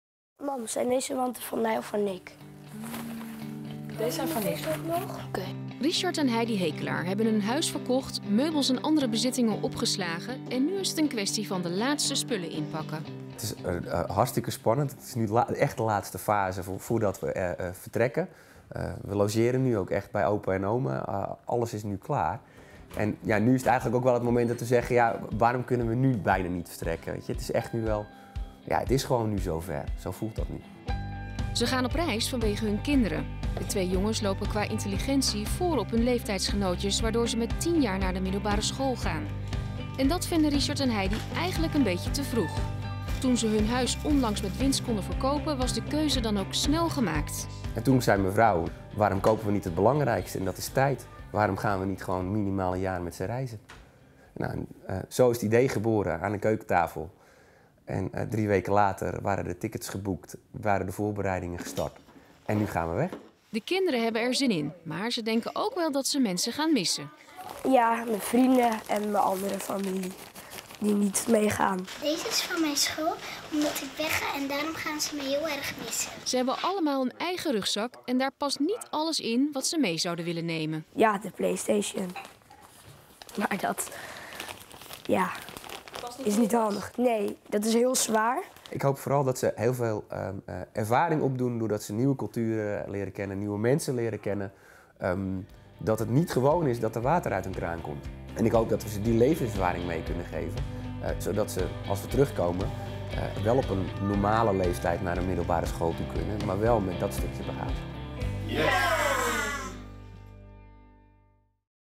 Goede voornemens? Deze familie gaat op wereldreis
De familie Hekelaar uit Hoogkarspel heeft wel een heel bijzonder voornemen voor het nieuwe jaar. Over twee dagen stapt het gezin met 3 kinderen op het vliegtuig voor een wereldreis. Maar liefst 1,5 jaar trekken ze erop uit en eigenlijk kunnen ze niet meer wachten.